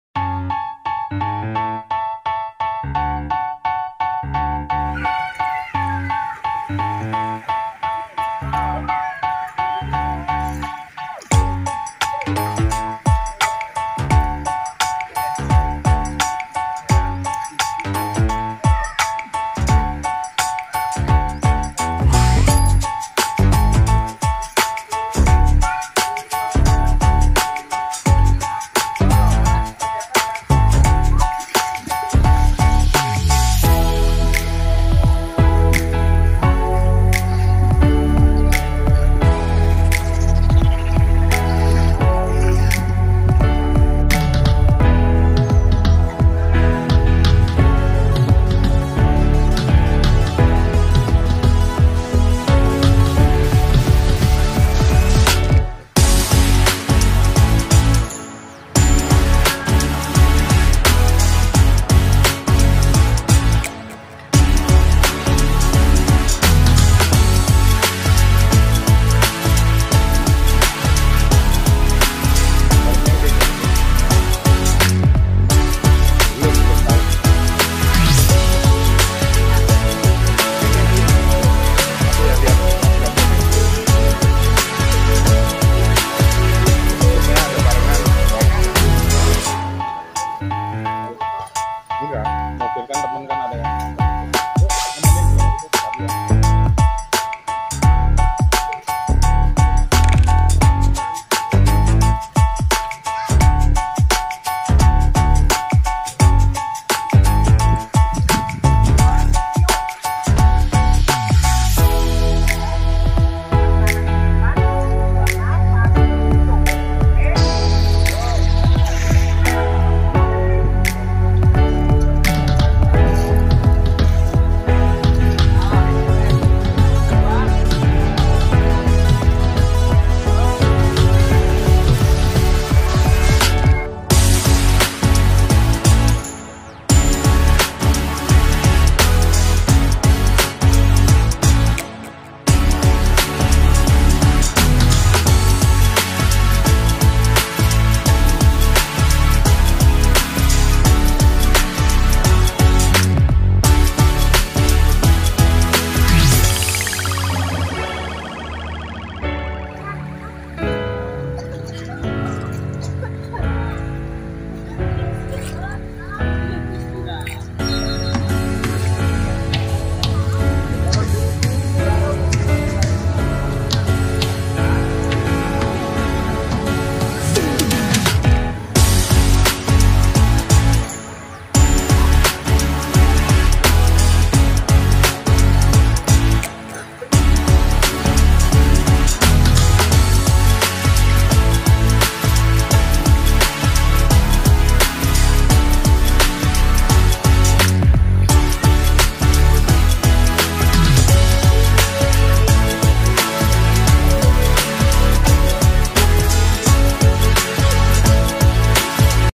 Cara Belajar Renang dengan Mudah di Trudgen Swimming Club
Belajar renang dengan mudah di Trudgen Swimming Club. jangan takut ketika anda belajar berenang yang terpenting ada pembimbing yang tepat.